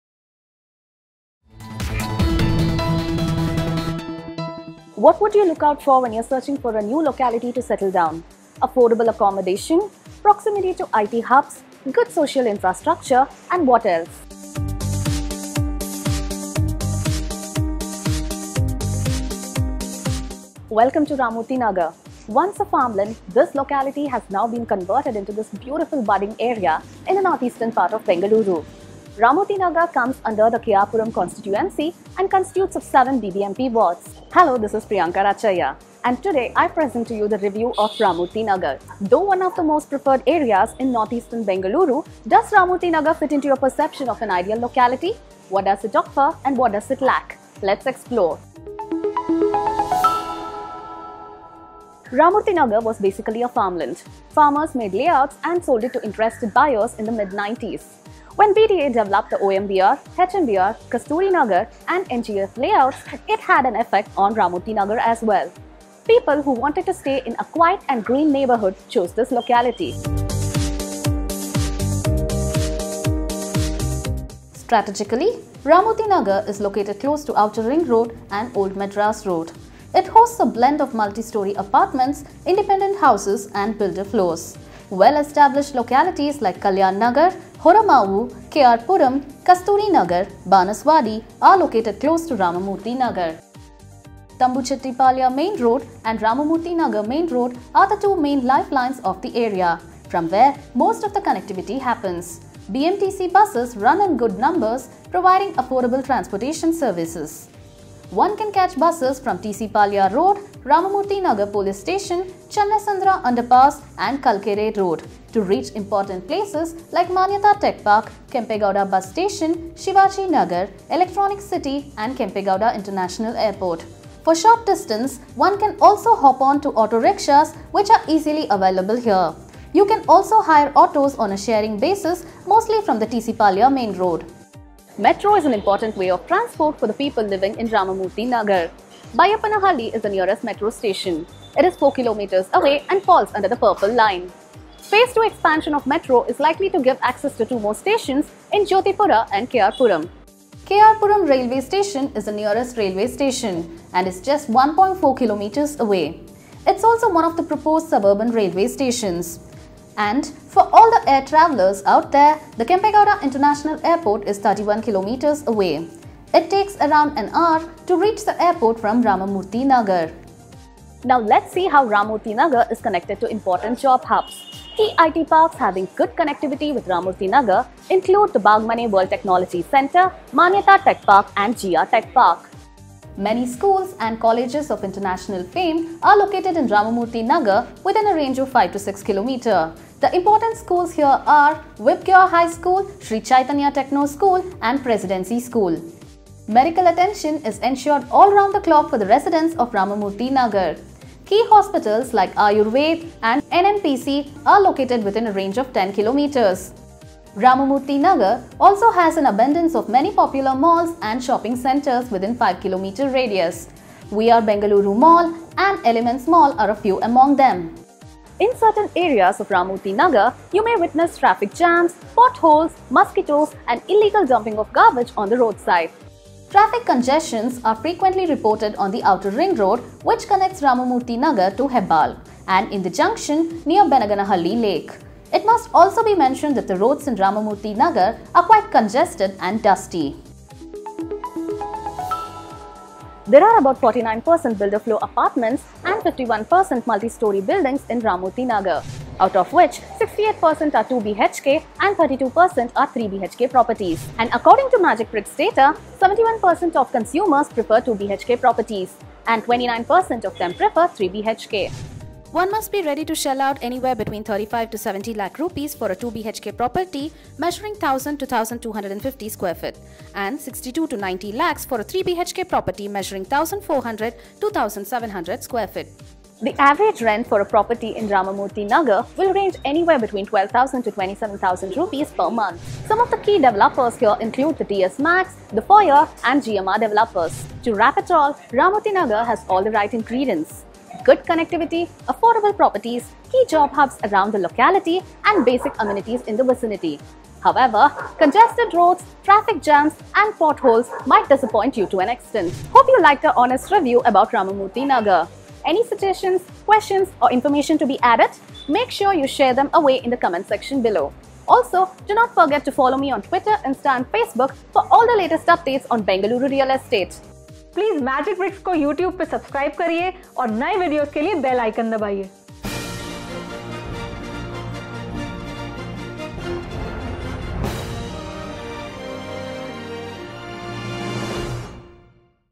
Ramamurthy Nagar, Bangalore Review: Price of Houses, Apartments, Villas, Plots, Commercial Property
The development of Kasturi Nagar and NGEF Layout significantly impacted Ramamurthy. People who want to live in a green and clean environment prefer to live in this locality. Strategically located, Ramamurthy Nagar is located close to OMR and ORR. It has a lot of multistorey apartments, independent houses, and builder floors.

Well-established localities like Kalyan Nagar, Horamavu, KR Puram, Kasturi Nagar, and Banaswadi are located close to Ramamurthy Nagar. BMTC buses are readily available, providing affordable transport services. It is also connected to Electronic City and Kempegowda International Airport. For a short distance, one can also rely on sharing autos and cabs, which are easily available here. The metro is an important means of transport for people living in Ramamurthy Nagar. Baiyappanahalli is the nearest metro station at 4 km, and KR Puram Railway Station is about 1.4 km away. Kempegowda International Airport is 31km away and takes around an hour to reach from Ramamurthy Nagar. Many tech parks like Bagmane World Technology Centre, Manyata Tech Park, and GR Tech Park can be easily accessed from here. Many schools and colleges are located in Ramamurthy Nagar within 5 to 6 km.

According to Magicbricks data, about 49% of properties in Ramamurthy Nagar are builder floors, while the remaining 51% are multistorey apartments. Apart from that, 2 BHKs have 68% supply and 3 BHKs have 32% supply. 2 BHKs are available in the price range of INR 35 lakh to 70 lakh, while prices for 3 BHKs are in the range of INR 62 to 90 lakh. The average rent for properties in Ramamurthy Nagar ranges between INR 12,000 - 27,000.